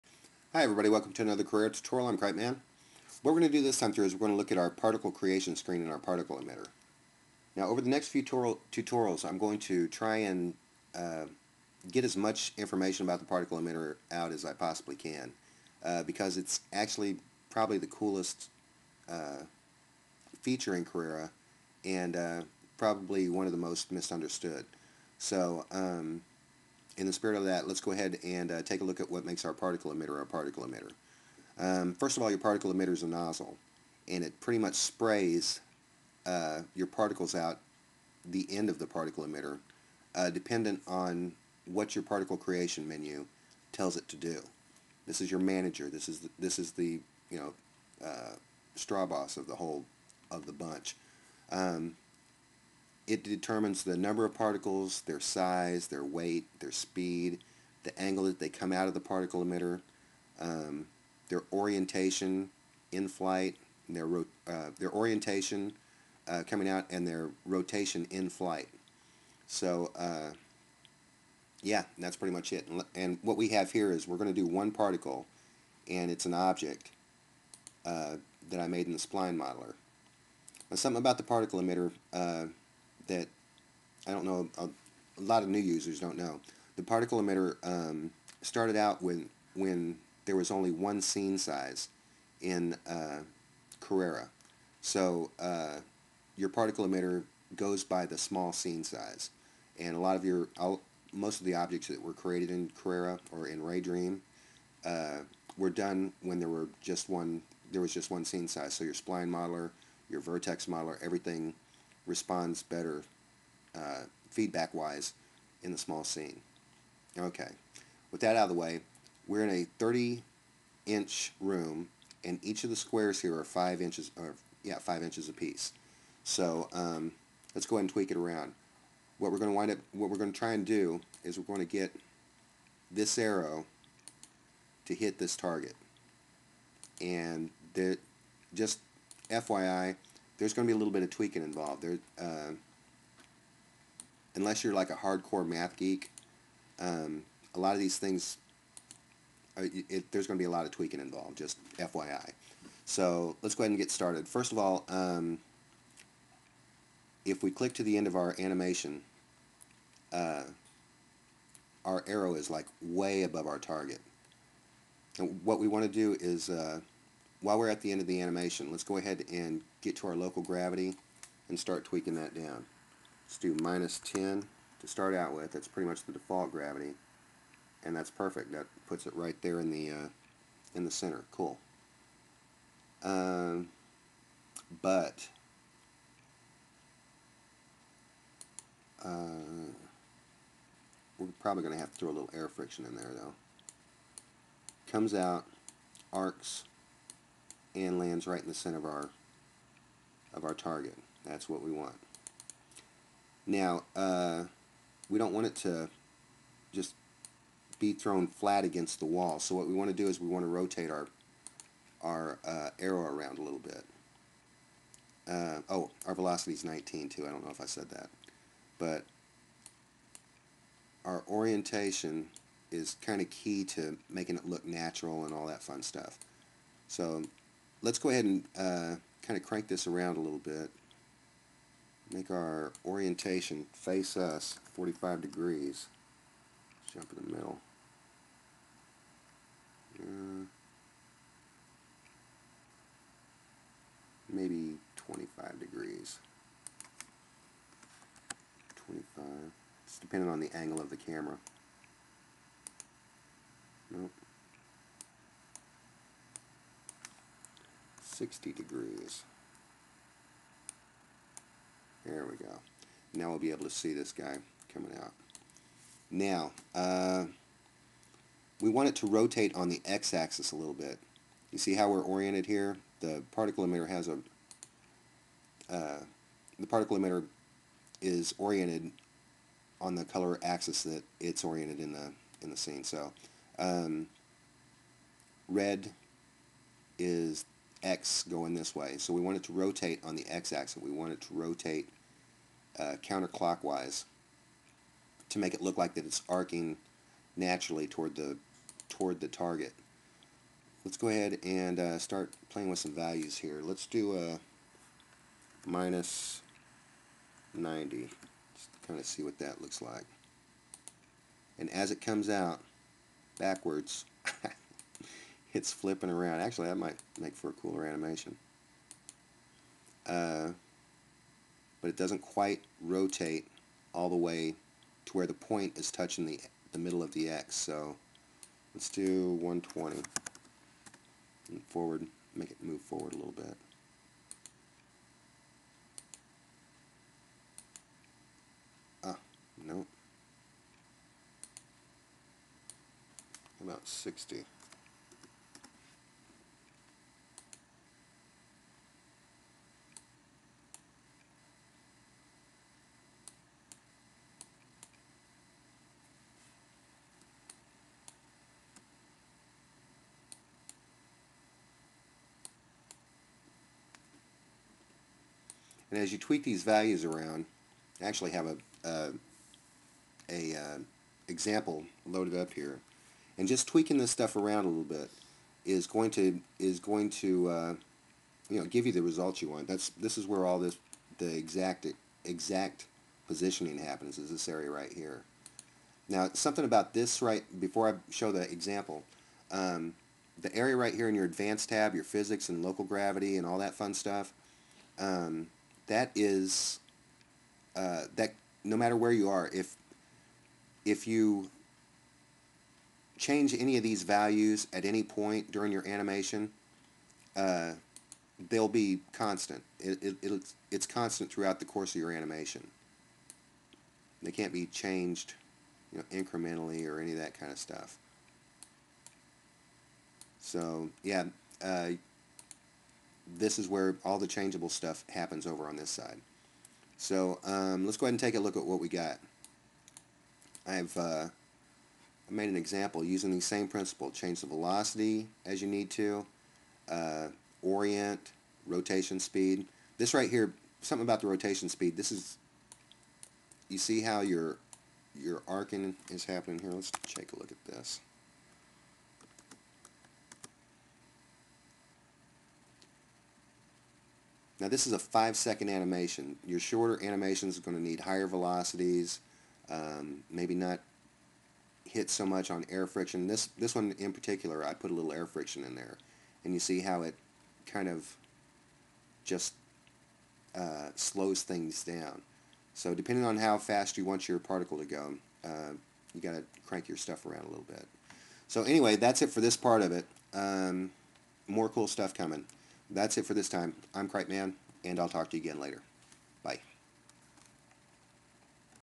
Particle Creation Menu
Part of a multi-part tutorial on the particle emitter.

This one focuses on the particle creation menu.